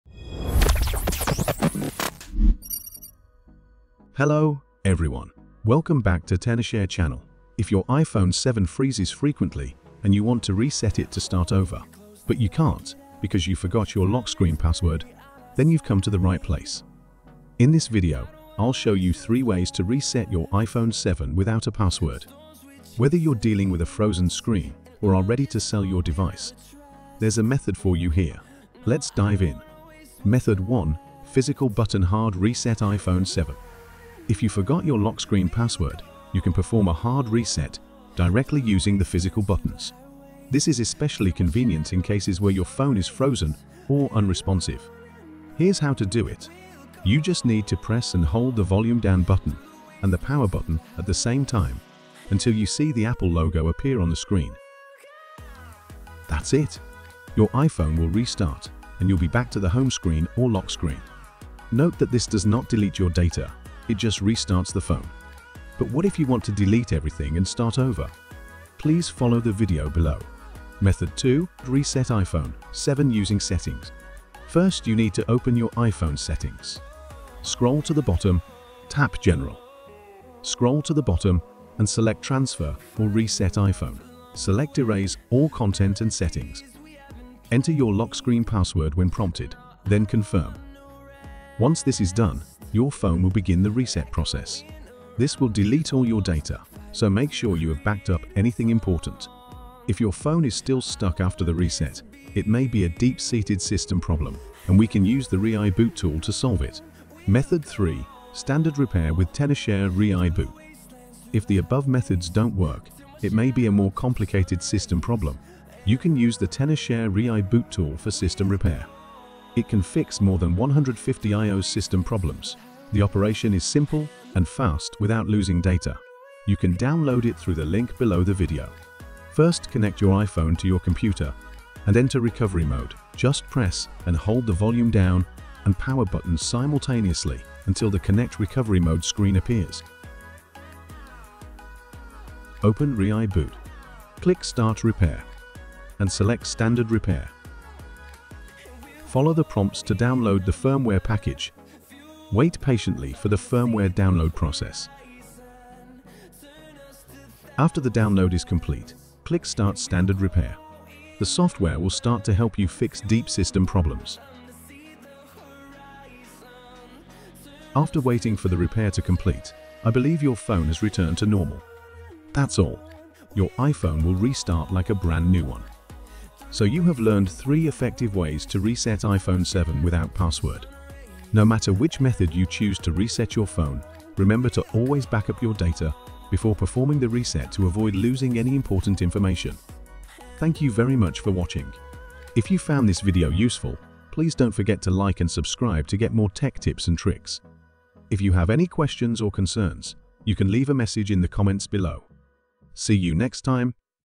How to Hard Reset iPhone 7 Without Password : 3 Easy Methods | 2025
If your iPhone 7 is freezing frequently, becoming a pain to use, and you're thinking about resetting it to start over, but find yourself stuck because you forgot your lock screen passcode, don't worry - you've come to the right place! This video will walk you through a simple and effective method to reset your device and get it back to normal, even without access to your passcode.

⏱ *TIMESTAMP*
00:00 intro
00:29 Method 1 : Physical Button Hard Reset iPhone 7
01:10 Method 2 :  Reset iPhone 7 using Settings
01:47 Method 3 : Standard Repair with Tenorshare ReiBoot

💡 *How to Hard Reset iPhone 7 Without Password*

⭐️Method 1: Hard Reset iPhone 7 Using Physical Buttons
1. Press and hold the Volume Down button and the Power button simultaneously.
2. Keep holding until the Apple logo appears on the screen.

Note: This method only restarts the device and does not erase any data.

⭐️Method 2: Reset iPhone 7 via Settings
1. Open the Settings app.
2. Tap General
3. Find the Transfer or Reset iPhone option.
4. Select Erase All Content and Settings.
5. Enter your lock screen password when prompted, then confirm.

Note : This method erases all data, so ensure you back up important information beforehand.

⭐️Method 3: Repair and Reset with Tenorshare ReiBoot
1. Connect your iPhone to a computer and put it in Recovery Mode (press and hold the Volume Down button and the Power button until the recovery screen appears).
2. Open the ReiBoot software, click Start Repair, and select Standard Repair.
3. Follow the prompts to download the firmware package.
4. Once the download is complete, click Start Standard Repair.
5. Wait for the repair process to finish. The device will restart like new.

💡Tip: Always back up your data before resetting your phone to avoid data loss.